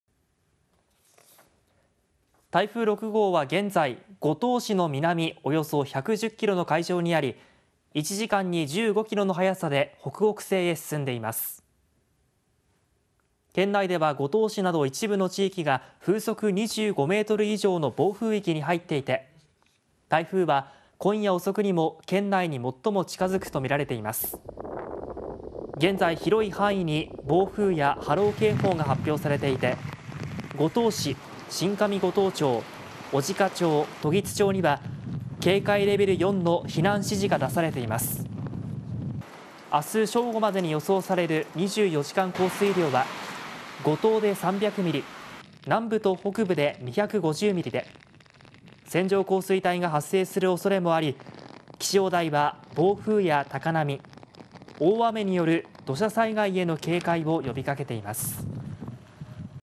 【台風6号】今夜遅くにも最接近 暴風へ厳重な警戒を【長崎】
台風6号は9日夜遅くにも長崎県内に最も接近する見込みで、気象台は暴風などへの厳重な警戒を呼びかけています。台風6号は９日午後4時現在、五島市の南およそ110キロの海上にあり、一時間に15キロの速さで北北西へ進んでいます。県内では五島市など一部の地域が、風速25メートル以上の暴風域に入っていて、台風は9日夜遅くにも県内に最も近づくと見られています。午後4時50分現在、広い範囲に暴風や波浪警報が発表されていて、五島市、新上五島町、小値賀町、時津町には警戒レベル4の避難指示が出されています。10日正午までに予想される24時間降水量は、
五島で300ミリ、南部と北部で250ミリで線状降水帯が発生するおそれもあり、気象台は暴風や高波、大雨による土砂災害への警戒を呼びかけています。